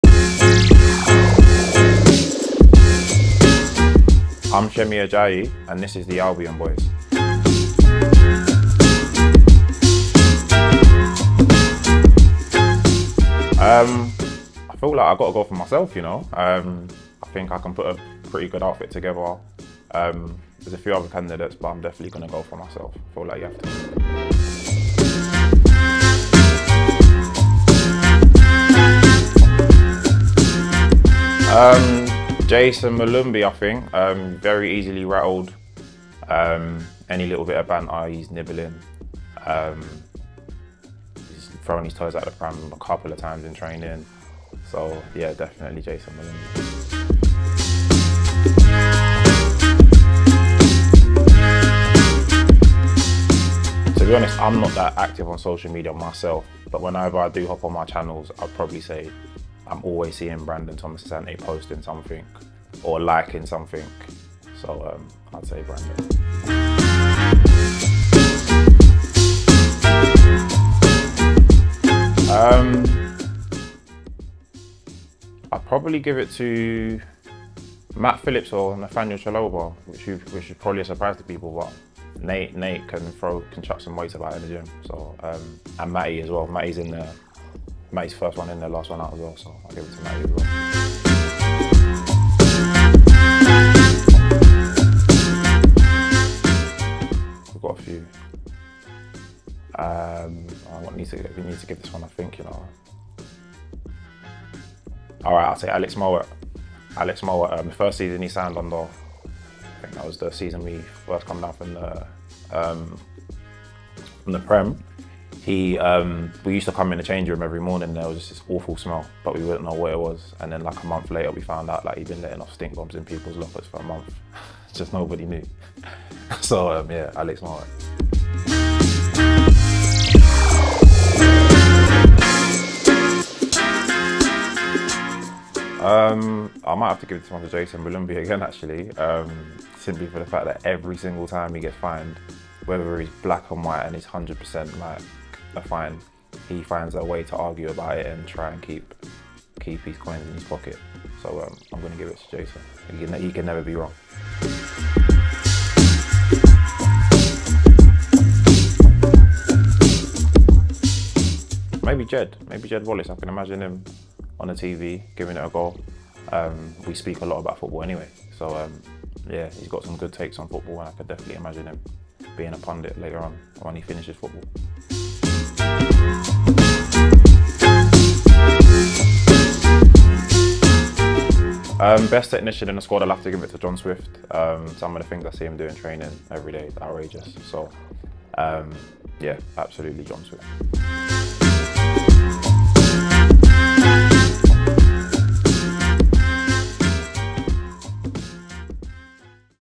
The Albion Boys with Semi Ajayi | Episode 5
Semi Ajayi is the latest Albion squad member to dish the dirt on his team-mates.